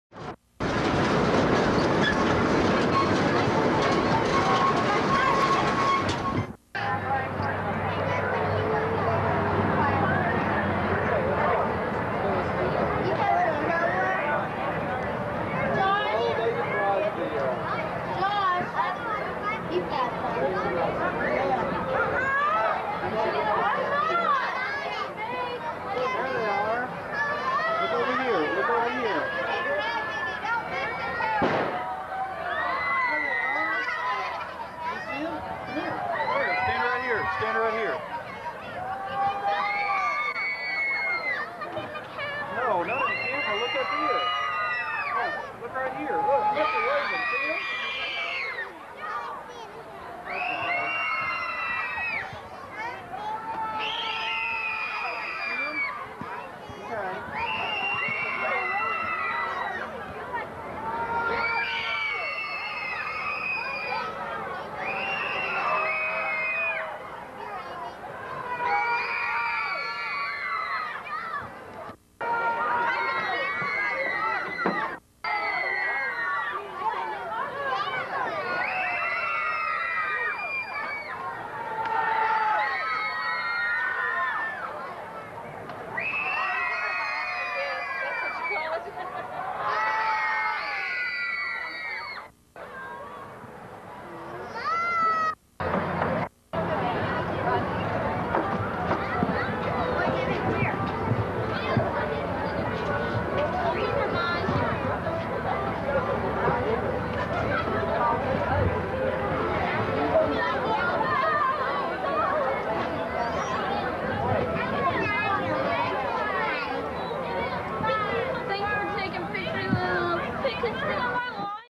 Prairie Schooner (Swinging Boat Ride) - September 1989 - Frontier City Ride Through
Prairie Schooner (Swinging Boat Ride) Ride Through POV Old Footage
Frontier City (Currently Owned by Six Flags)
Oklahoma City, OK
September 1989